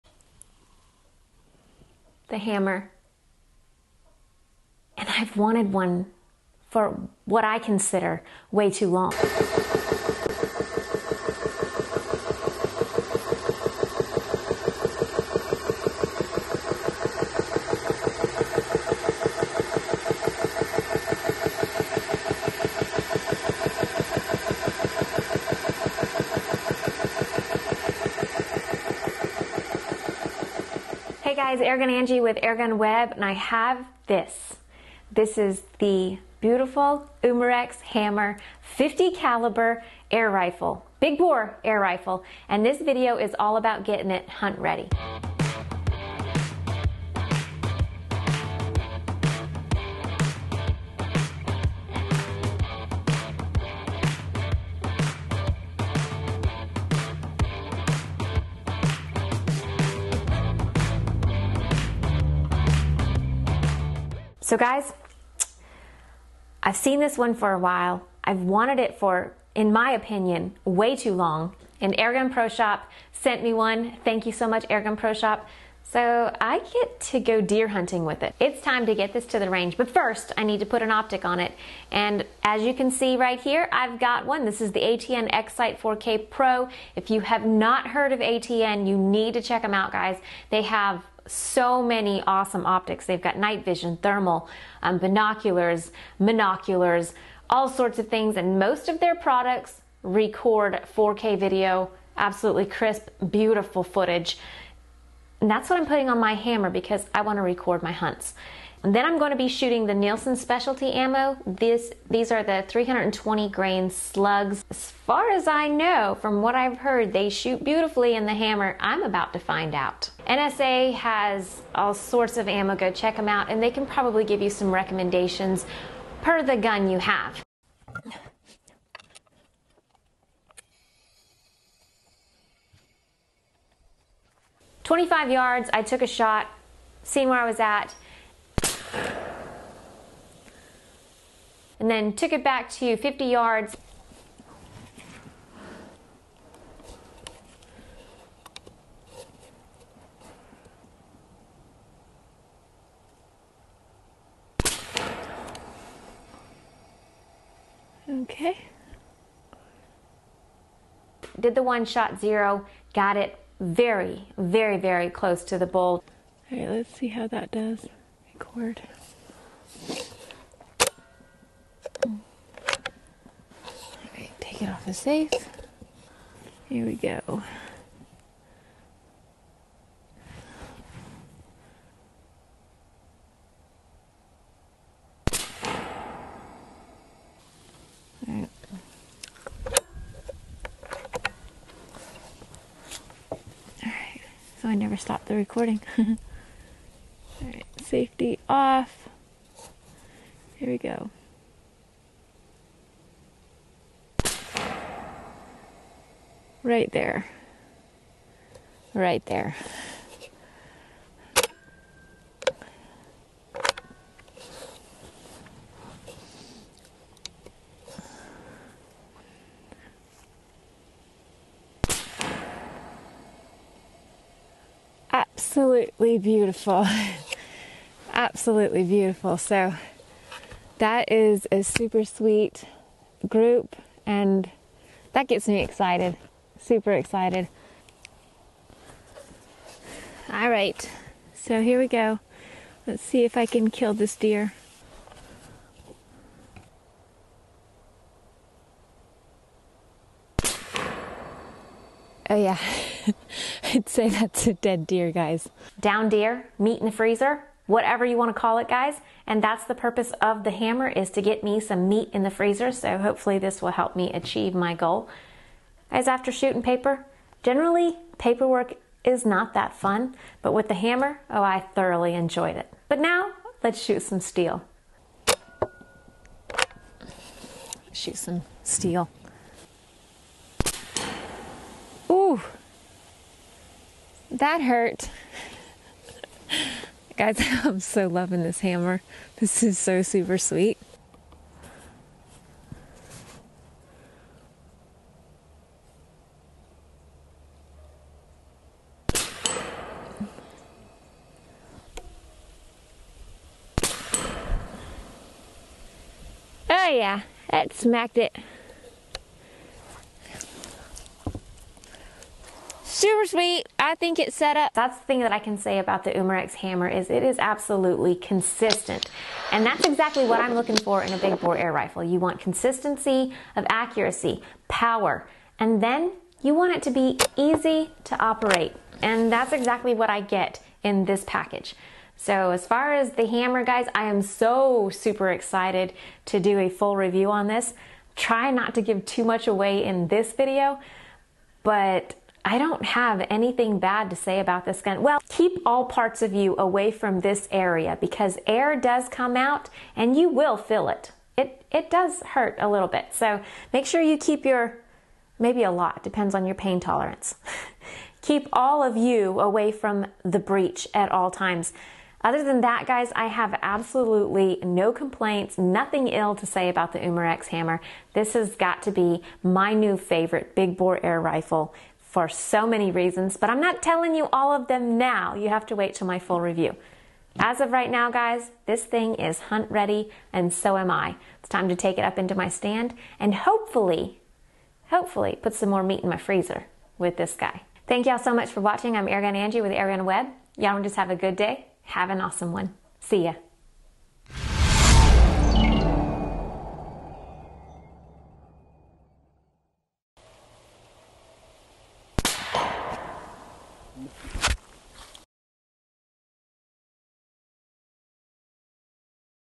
UMAREX HAMMER – Hunt Ready
Time to get my Umarex Hammer Hunt Ready with the ATN X-Sight 4K Pro and some Nielsen Specialty Ammo.